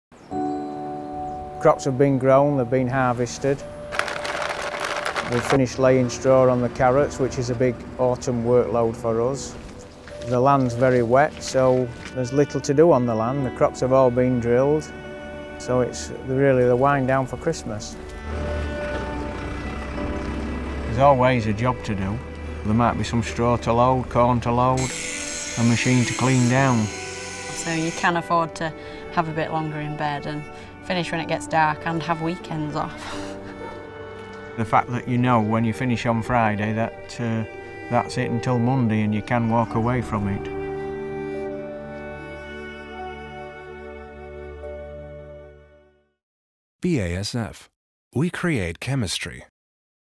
BASF Farmer Stories I Join an Arable Farming Family During Holiday Season
BASF visited an arable farming family in the UK to see what their holiday season looks like.

Copyright:
glare-hope-homecoming-youthful